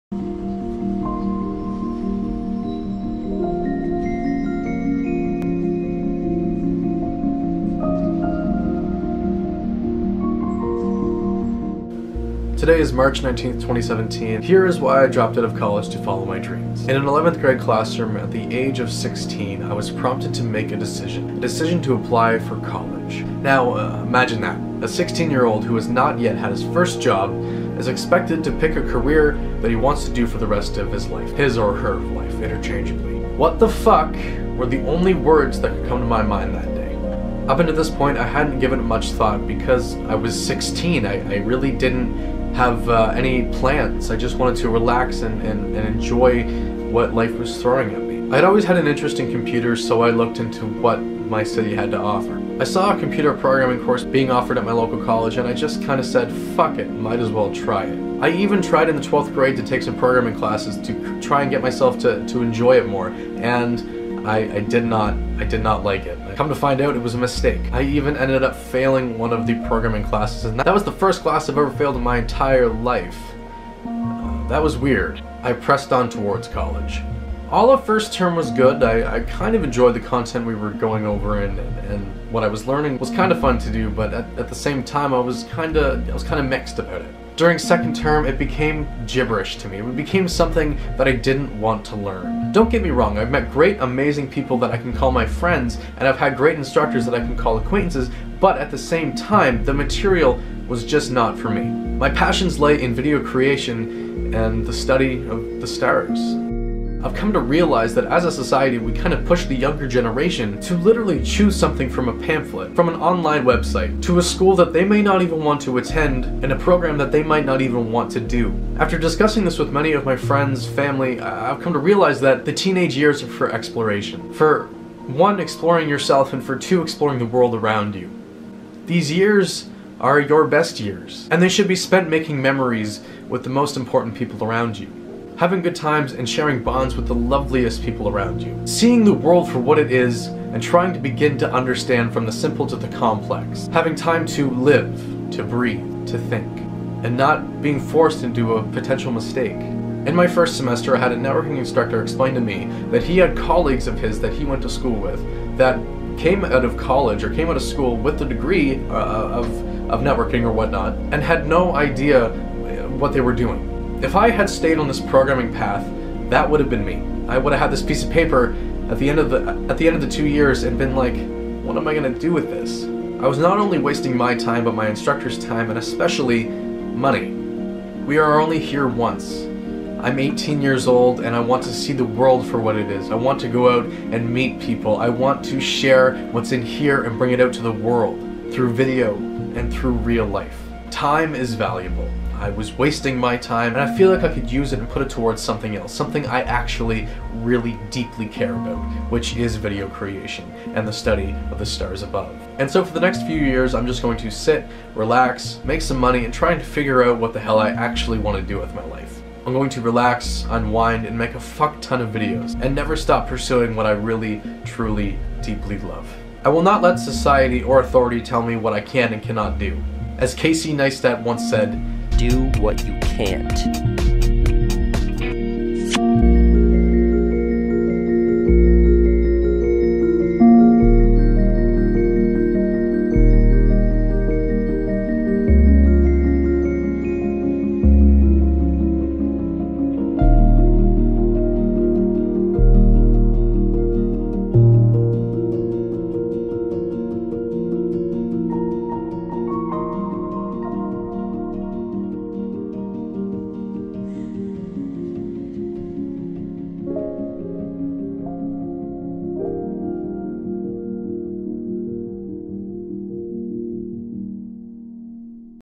DROPPING OUT OF COLLEGE TO FOLLOW MY DREAMS
I have dropped out of college to follow my dreams. Watch for an explanation as to why.